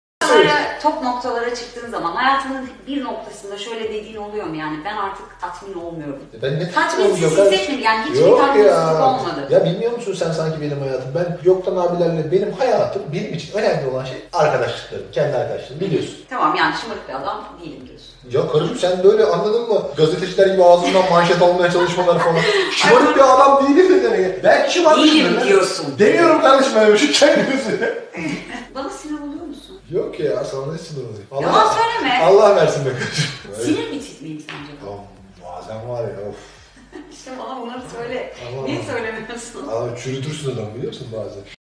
12 Mayıs 2021
Arda Turan, eşi Aslıhan Doğan Turan’ın YouTube kanalına konuk oldu. Karı koca arasında ‘şımarık’ atışması yaşandı!